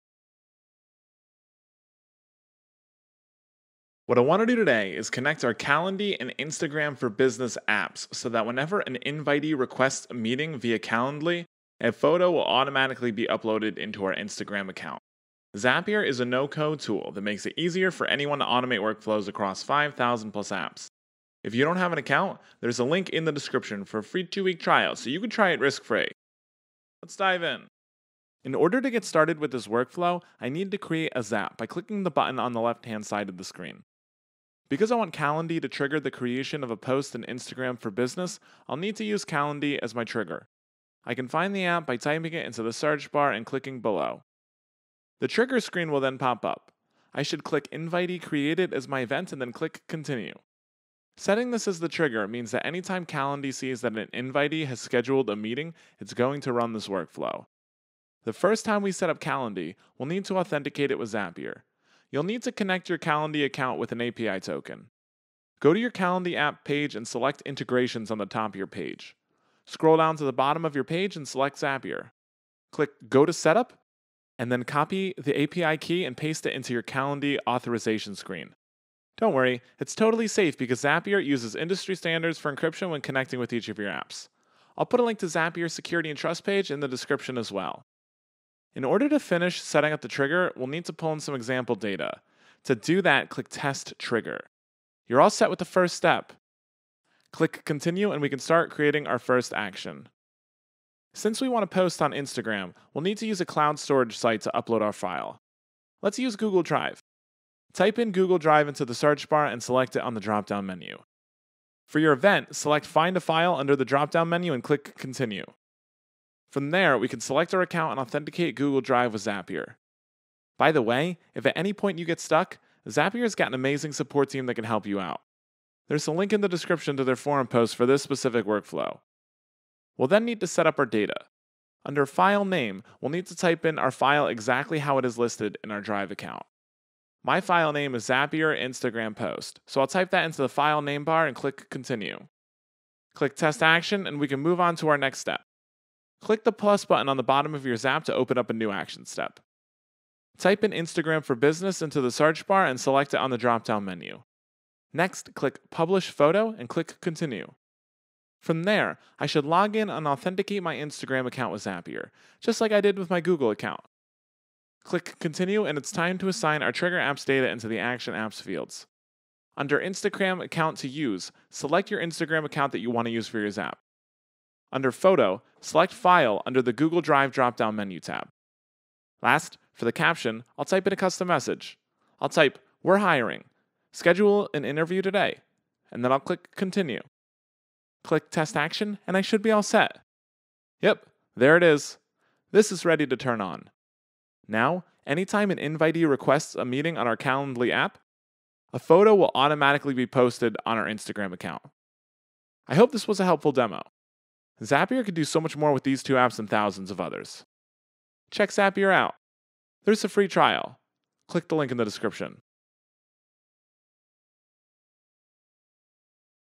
How to Sync Calendly to Instagram for Business- Easy Integration Tutorial
Do you want to Sync Calendly to Instagram? Then this easy integration tutorial is for you!